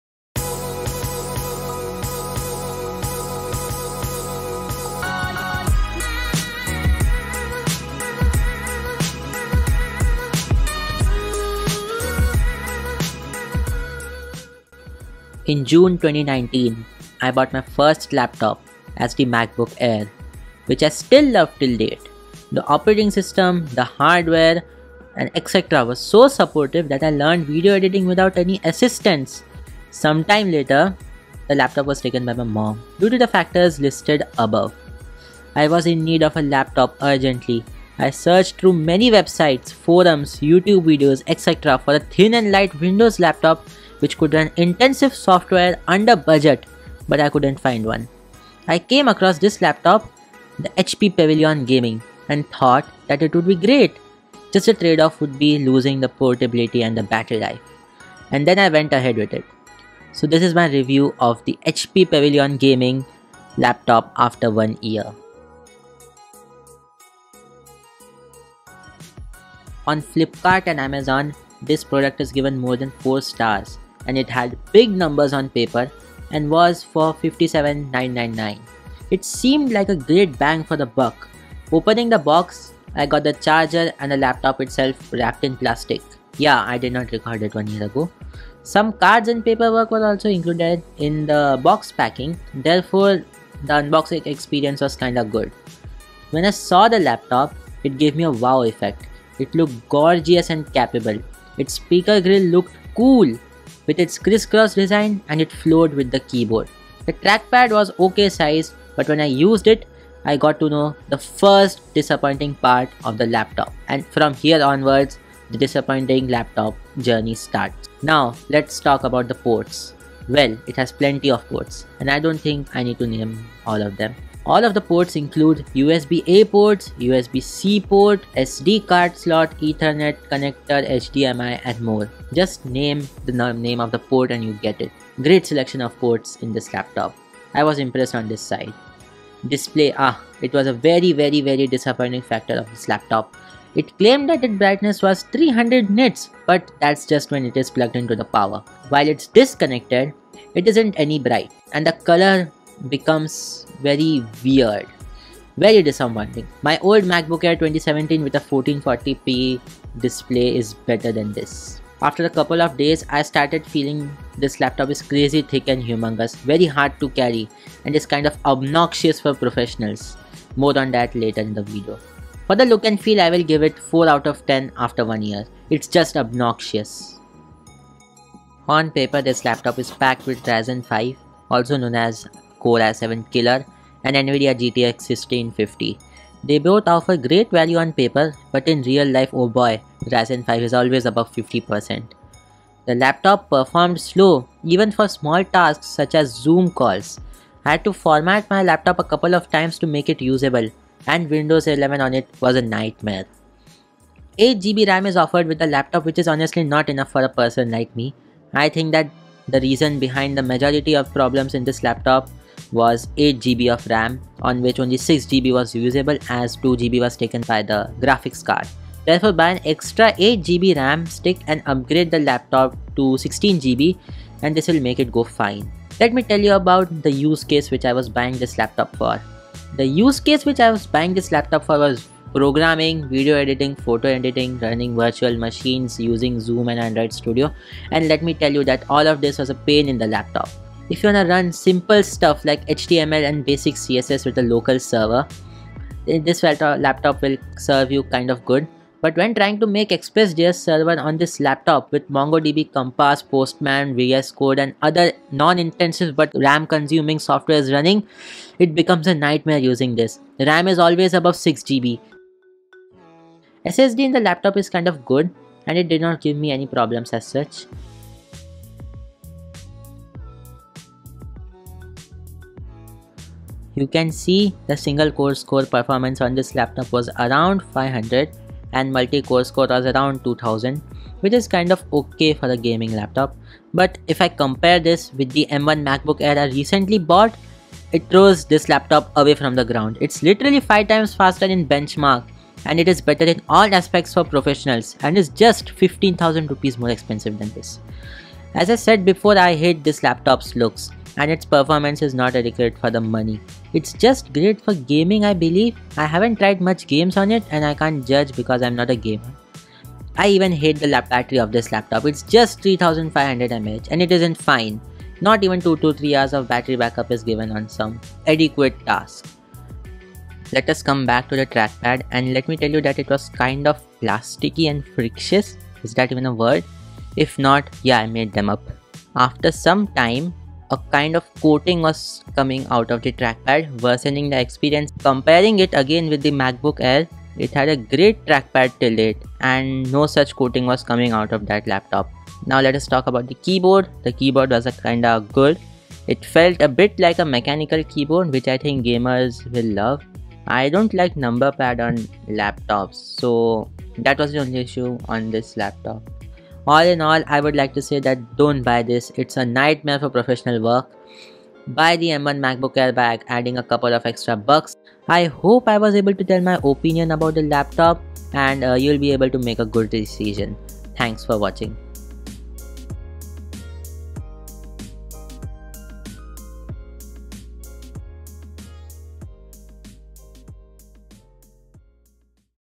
HP pavilion gaming didn't satisfy me | HP pavilion gaming 3550h review after an year | Zaid Ajani
A year ago due to some "unexpected" situations, I had to buy a new laptop in which I could do programming, video editing, photo editing, and some normal tasks like using zoom and google chrome. Came across this laptop and bought it. To be honest the experience was so bad that I had to sell it after using it for a year ( no exaggeration ) For more information about the laptop, watch this video till the end.